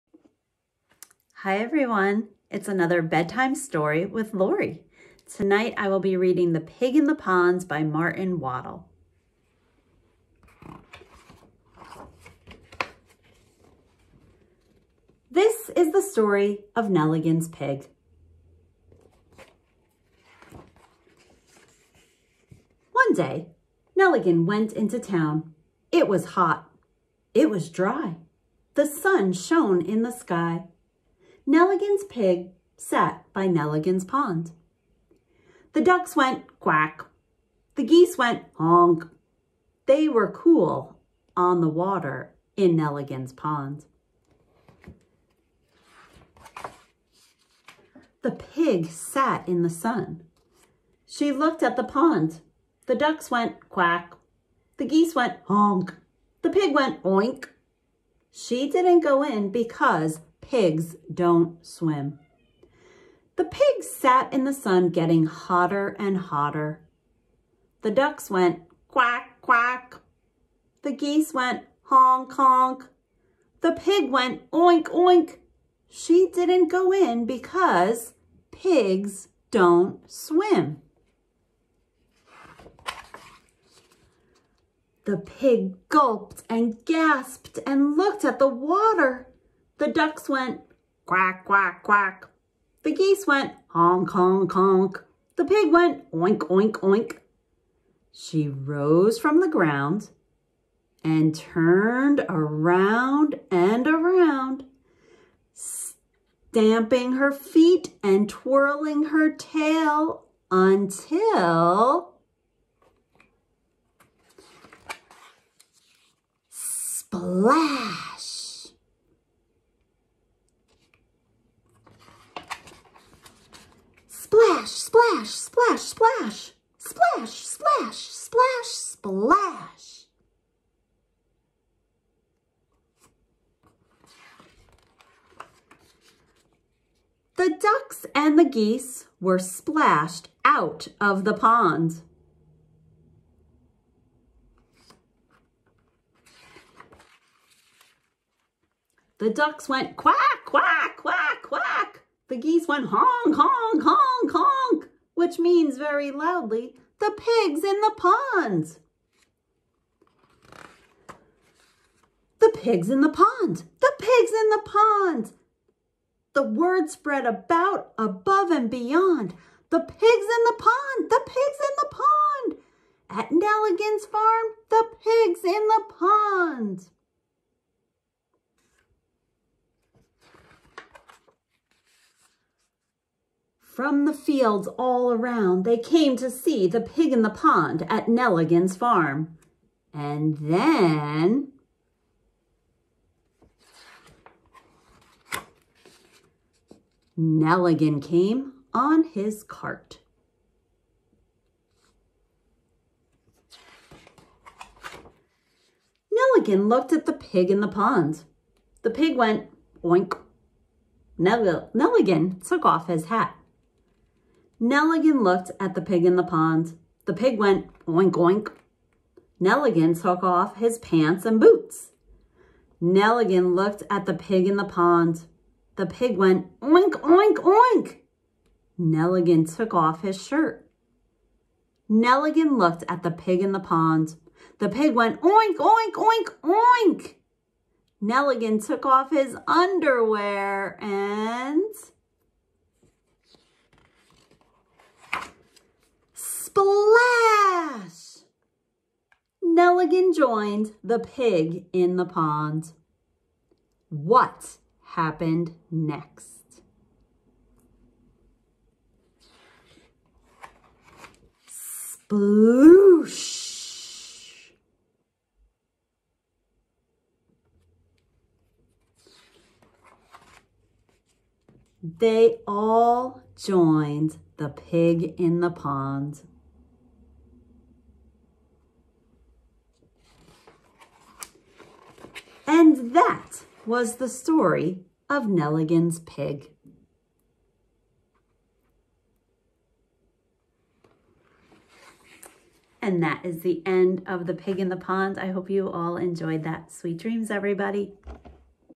Bedtime Story with Lori - The Pig in the Pond by Martin Waddell
Video was made as a way to help support parents with young children during the Covid-19 crisis. Video will be removed by June 30, 2020.

Parents with young children: do you need five minutes? I see you and how you are trying to deal with all of the sudden shifts in normal life.

Bedtime Story with Lori is here for you! Turn this video on before bedtime and let your child listen to me read a story while you take a few minutes to breathe, drink water, stretch, or whatever you can do in a short time to take care of YOU.